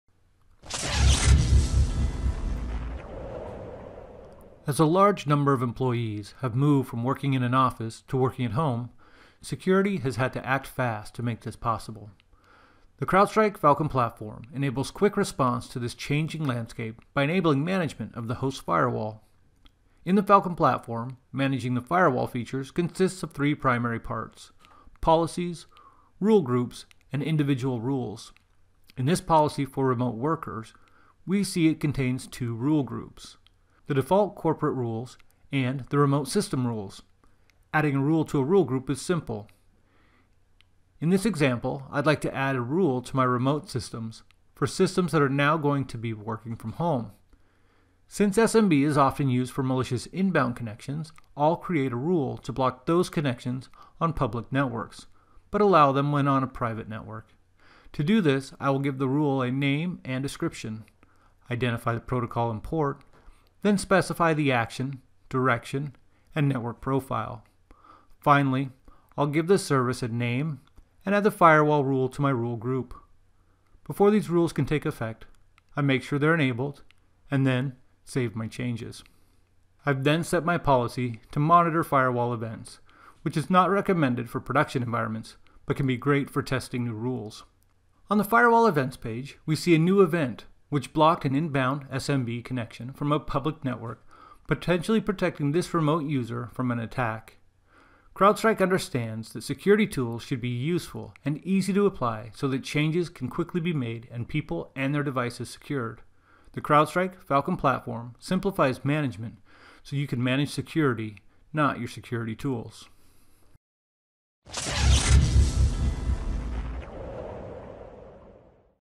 Host Firewall Protection for a Remote Workforce
In this demo we'll look at some of the changes facing organizations who are adapting to a remote workforce and the roll that their host firewall plays in those security changes.